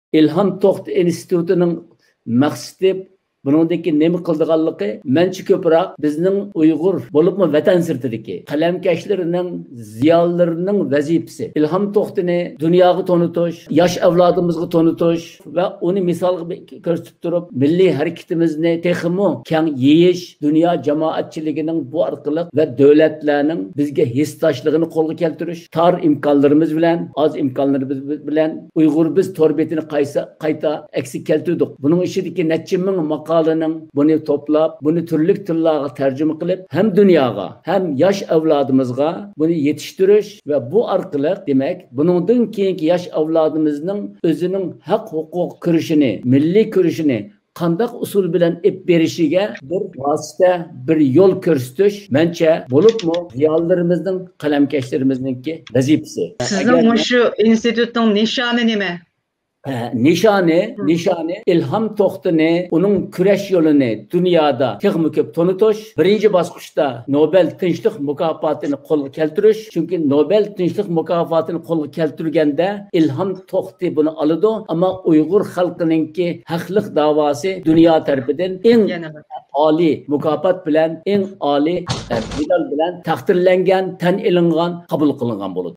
ئەنۋەر جان: «ئىلھام توختى ھەرىكىتىنىڭ مەقسىتى ئىلھام توختىنى دۇنياغا، ياش ئەۋلادىمىزغا تونۇتۇش»
ئەنۋەر جان: «ئىلھام توختى ھەرىكىتىنىڭ مەقسىتى ئىلھام توختىنى دۇنياغا تونۇتۇش، ياش ئەۋلادىمىزغا تونۇتۇش ۋە ئۇنى مىسالغا كۆرسۈتۈپ تۇرۇپ، مىللى ھەرىكتىمىزنى تېخىمۇ كەڭ يېيىش. دۇنيا جامائەتچىگى ۋە دۆلەتلەرنىڭ بۇ ئارقىلىق بىزگە ھېسداشلىقىنى قولغا كەلتۈرۈش.»